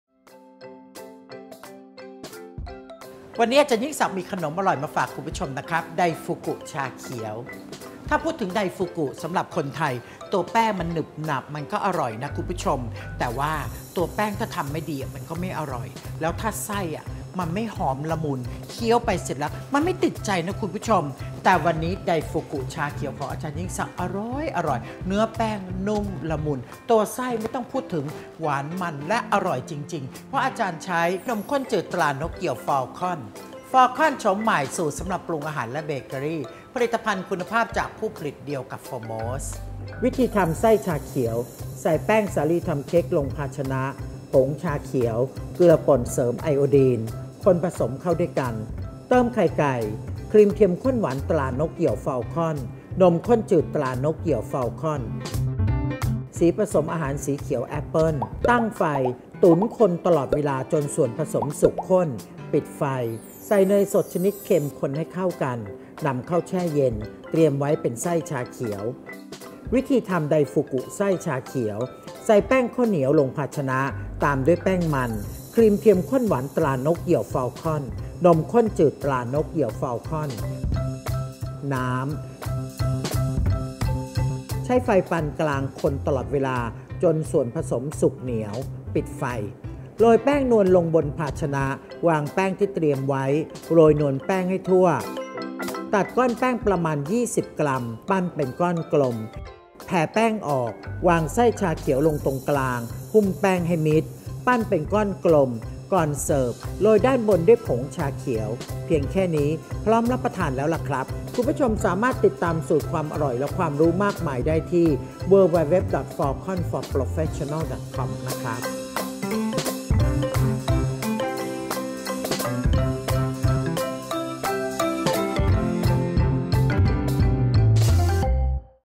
ไดฟูกุไส้ชาเขียว สูตรเด็ด ทำง่าย สไตล์ "อ.ยิ่งศักดิ์"
ไดฟูกุไส้ชาเขียว
Daifuku green tea filling

ผลิตภัณฑ์ : นมข้นจืด ตรานกเหยี่ยว ฟอลคอน และครีมเทียมข้นหวาน ตรานกเหยี่ยว ฟอลคอน Sterilized recombined flavored milk for cooking and bakery (Falcon brand), Sweetened condensed non-dairy creamer (Falcon brand)

สิ่งที่ต้องเตรียมสำหรับ (ไดฟุกุไส้ชาเขียว)

แป้งข้าวเหนียว 250 กรัม
แป้งข้าวโพดคั่วจนสุกร้อน สำหรับทำแป้งนวล
ไส้ชาเขียว สำหรับสอดไส้

1.ใส่แป้งข้าวเหนียว แป้งมัน ครีมเทียมข้นหวาน ตรานกเหยี่ยว ฟอลคอน
นมข้นจืด ตรานกเหยี่ยว ฟอลคอนและน้ำลงในภาชนะ คนให้เข้ากัน
 2.ตั้งไฟปานกลาง คนตลอดเวลาจนส่วนผสมสุกใส ปิดไฟ  เตรียมไว้
3.โรยแป้งนวลลงบนภาชนะ วางแป้งที่เตรียมไว้ โรยแป้งนวลให้ทั่ว
คลึงแป้งออก ตัดแบ่งเป็นท่อน ประมาณ 20 กรัม ปั้นเป็นลูกกลม
4.แผ่แป้งออก ใช้สกู๊ปไอศกรีมตักไส้ชาเขียวลงตรงกลาง หุ้มแป้งให้มิด
ปั้นเป็นก้อนกลม จัดใส่ภาชนะ ก่อนเสิร์ฟโรยด้านบนด้วยผงชาเขียว
จัดรับประทานคู่กับชาร้อนๆ

สิ่งที่ต้องเตรียมสำหรับ (ไส้ชาเขียว)

ผงชาเขียว 2 ช้อนโต๊ะ
สีผสมอาหารสีเขียวแอปเปิ้ล ½ ช้อนชา

1.ใส่แป้งสาลีทำเค้ก ผงชาเขียวและเกลือป่นเสริมไอโอดีน ลงในภาชนะ
คนผสมเข้าด้วยกัน
2.เติมไข่ไก่ ครีมเทียมข้นหวาน ตรานกเหยี่ยว ฟอลคอน นมข้นจืด
ตรานกเหยี่ยว ฟอลคอนและสีผสมอาหารสีเขียวแอปเปิ้ล
คนจนส่วนผสมเข้ากัน ด้วยตะกร้อมือ นำขึ้นตั้งไฟตุ๋น
คนตลอดเวลาจนส่วนผสมสุกข้น ปิดไฟ
3.ใส่เนยสดชนิดเค็ม คนให้เข้ากัน นำเข้าแช่เย็น
เตรียมไว้เป็นไส้ชาเขียว

▲Line @yingsakfood